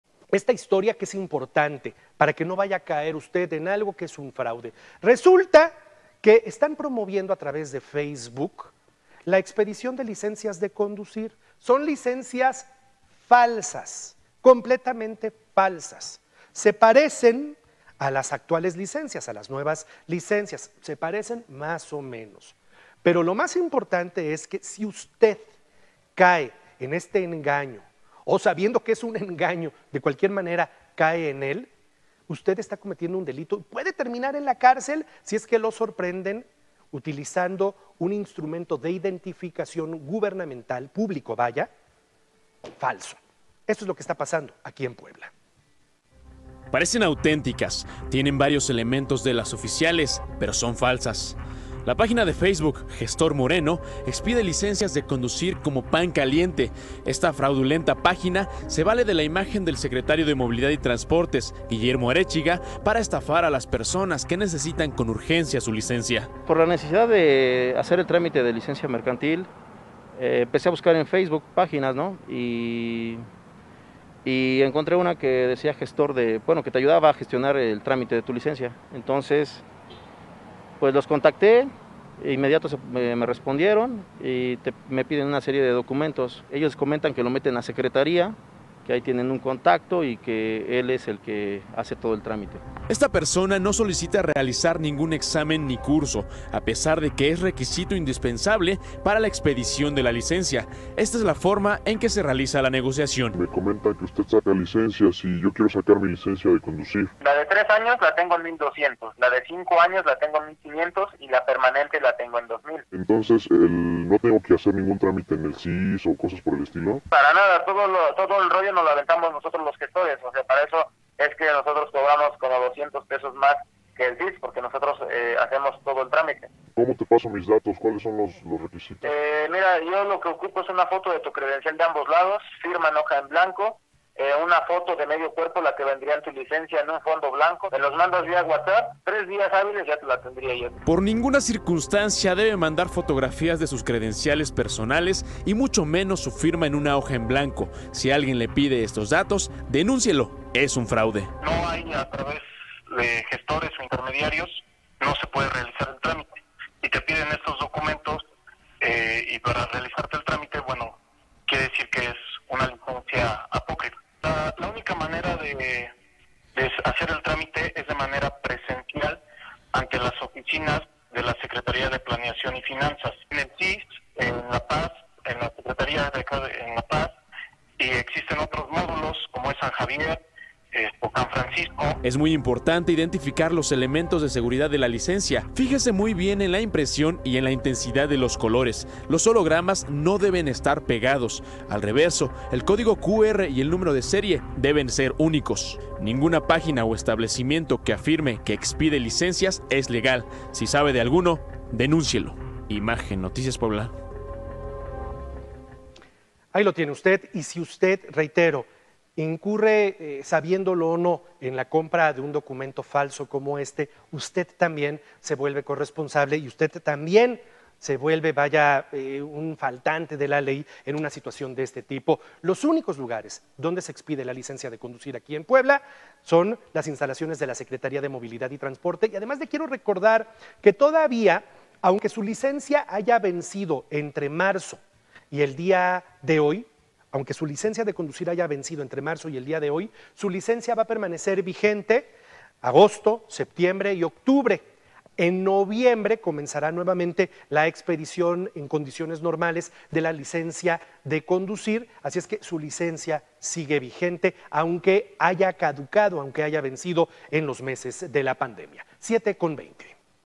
Licencias Falsas || Noticias con Juan Carlos Valerio
¡Tenga mucho cuidado! Hay personas que se están aprovechando de los poblanos que les urge tramitar su licencia de conducir y les están entregando licencias falsas.